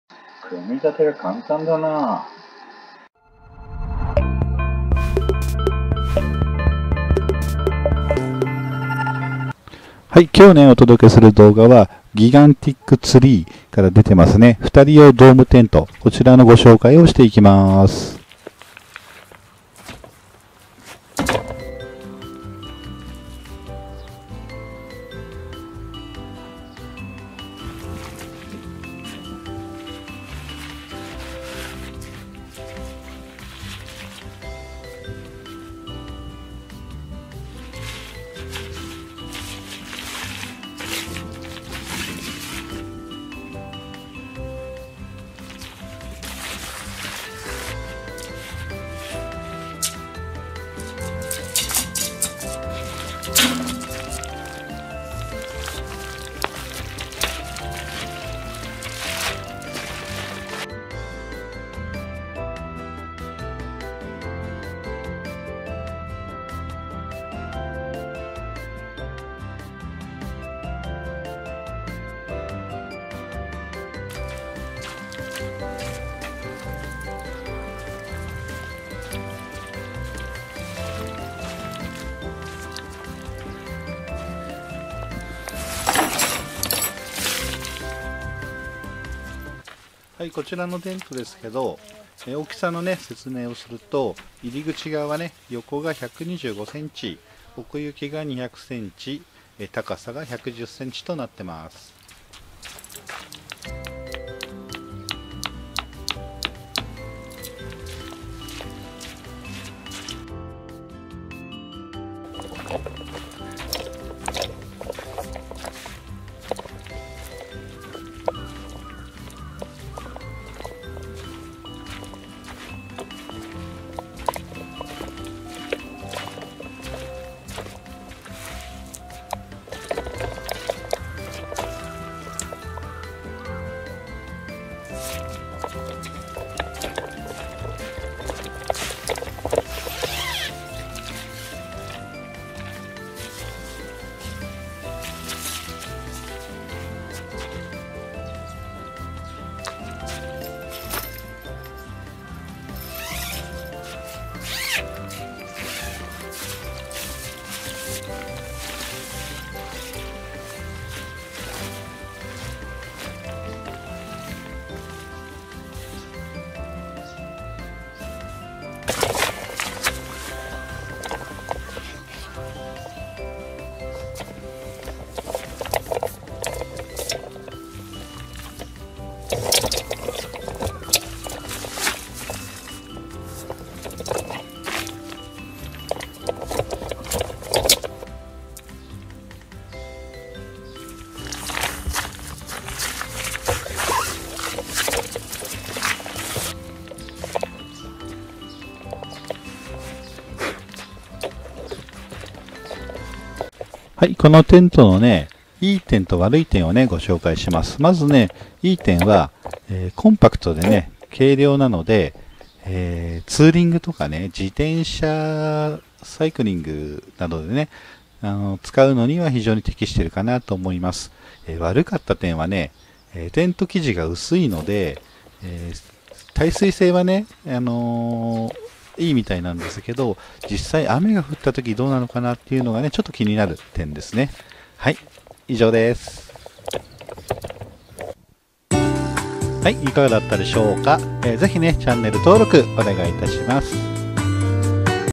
【テント設営】ギガンティックツリー２人用ドームテント開封設営レビュー
ギガンテックツリーの二人用テントのレビューです。
ソロキャンプやツーリングまたはサイクリングキャンプにいいかもですね